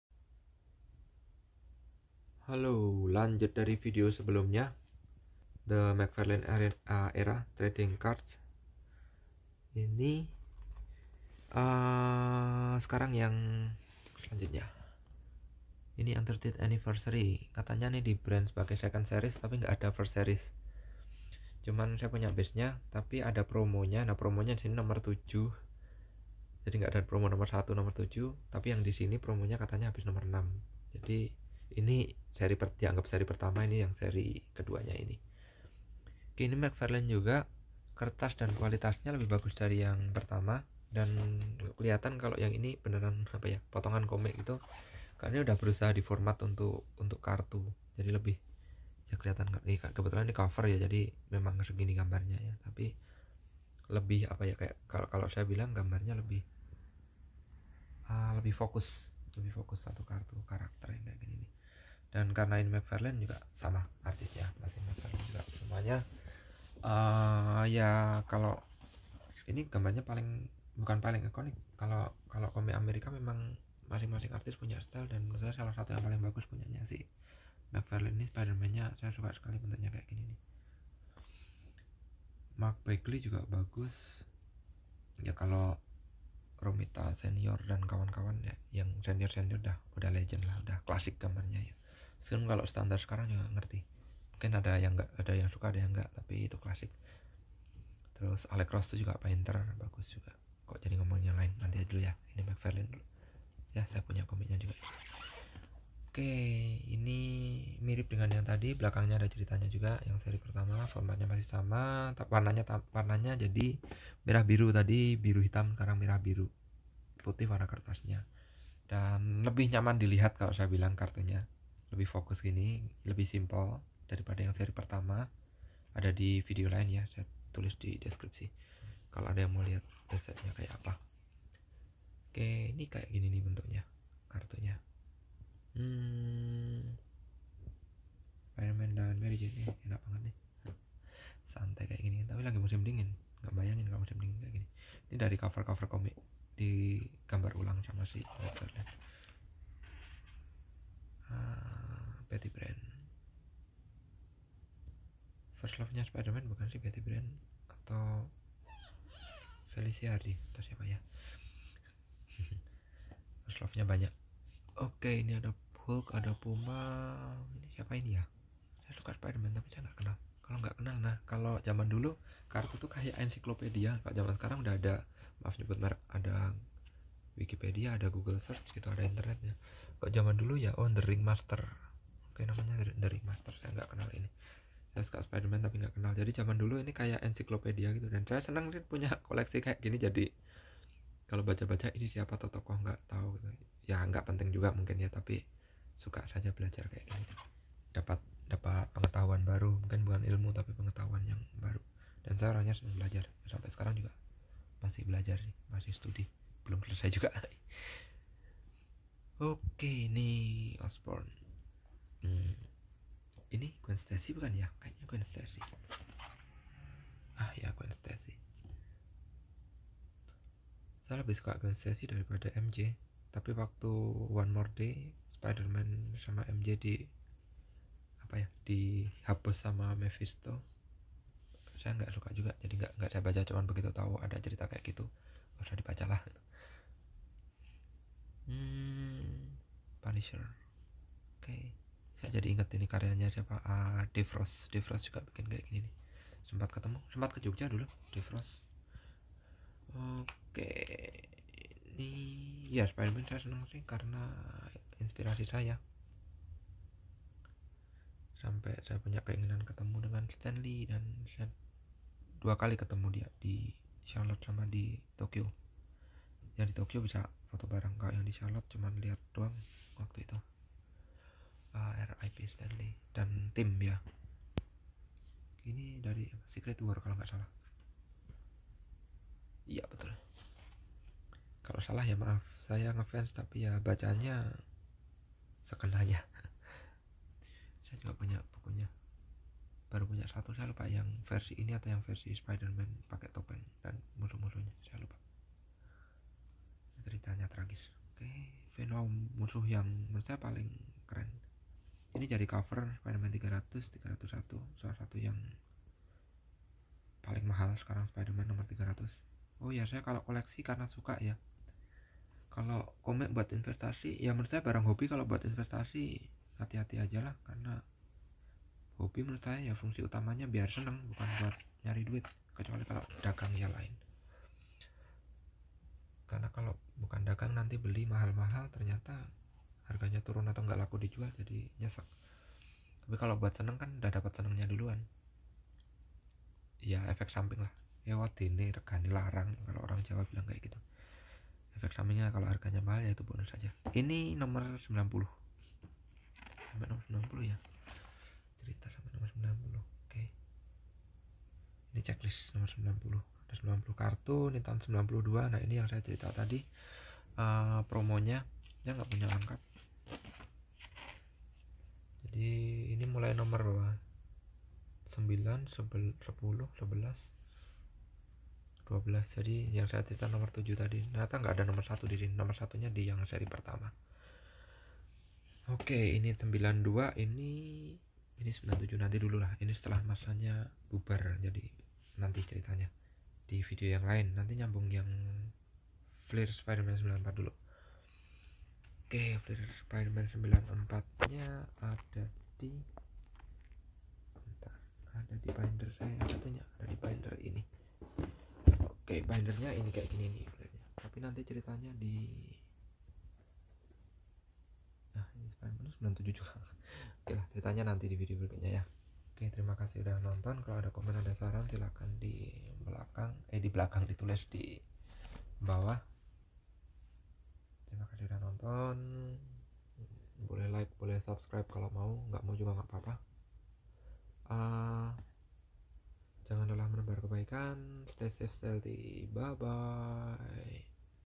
1992 Spider-Man 30th anniversary trading cards Mc Farlane series II
Kartu seri kedua, namun secara formal ga pernah ada seri pertama. Kartu ini lanjutan dari 1992 comic images Spider-Man McFarlane era Kartunya masih sama, menggunakan art dari komik karya McFarlane, namun dengan desain yang lebih fokus dan lebih dipersiapkan untuk kartu. Kualitas kertas, cetak dan finishing juga lebih bagus dari seri pertama. Penggemar komik 90s yang koleksi kartu dan fokus pada artis komik kayaknya perlu melirik kartu ini sebagai koleksi dan temennya, 1992 X-Men yang dikerjakan oleh Jim Lee Dua artis ini menonjol dan kontroversial, karena di puncak karirnya mengambil langkah yang nekat, meski dalam perkembangannya... well, bisa cari sendiri cerita mengenai beliau berdua. Back to kartu aja, sila cek videonya...